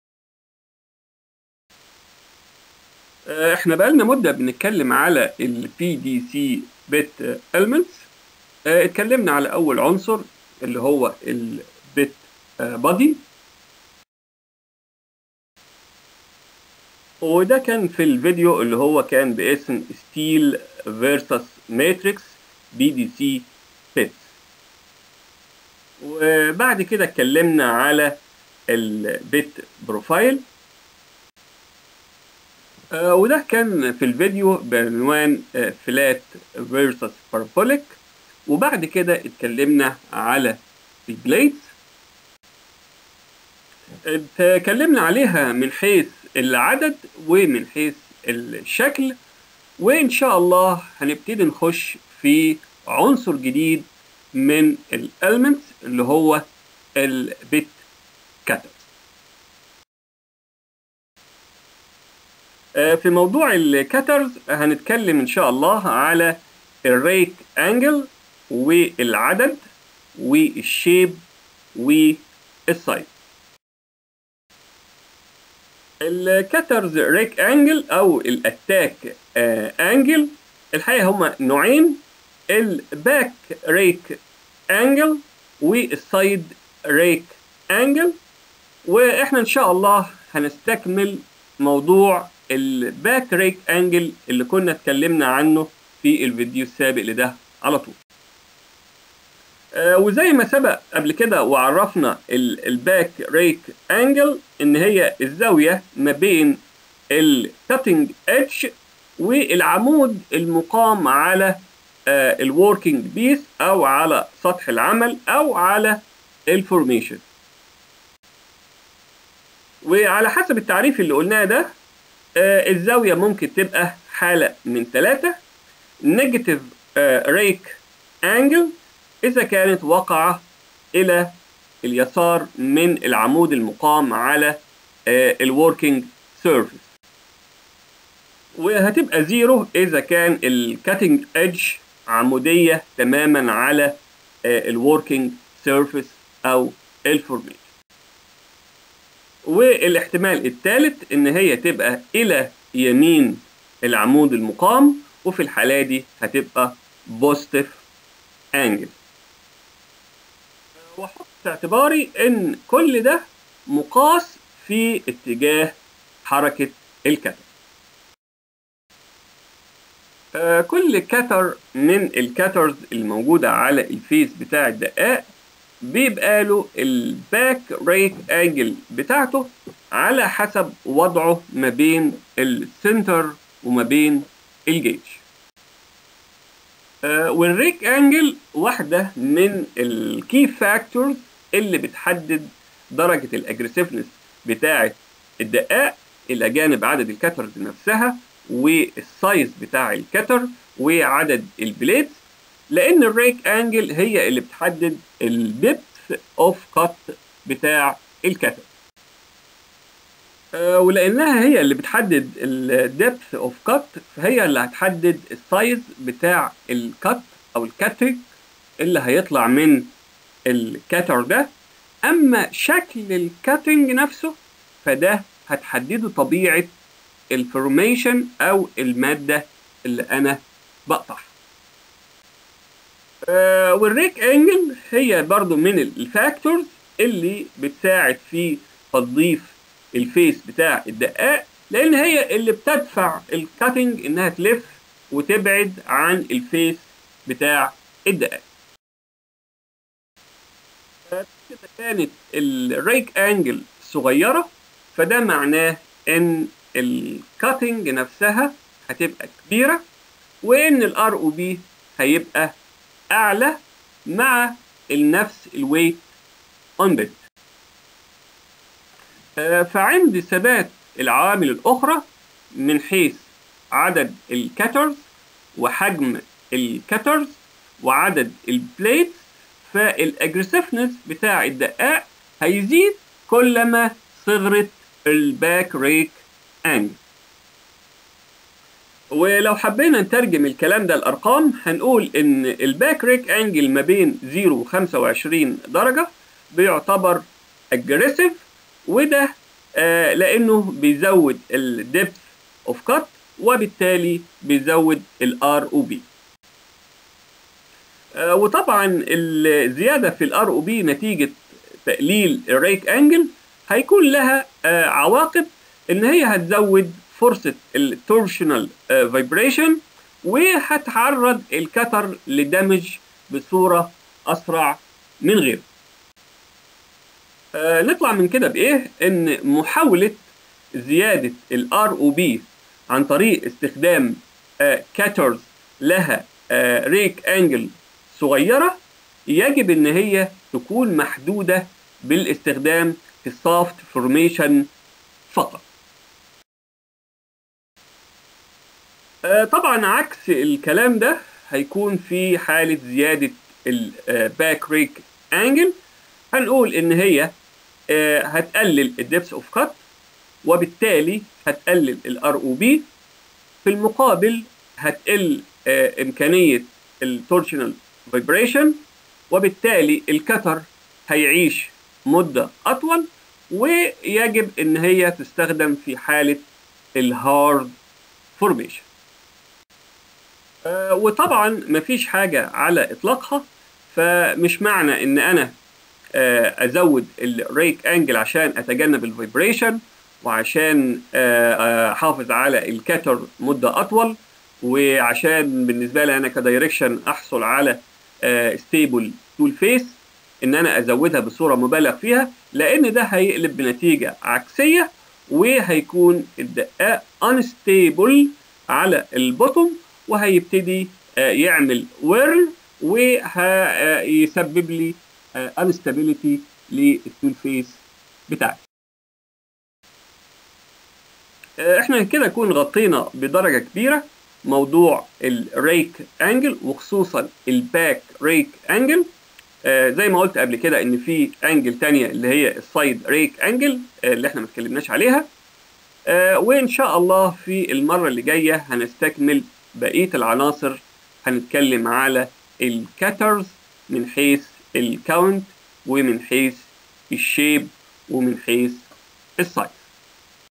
PDC Bits Cutters: Back Rake Angle 2
Further explanation of the PDC cutters back rake angle and the importance of its value in determining the degree of the PDC bit aggressiveness and the related items such as ROP, Vibration and Toolface stability.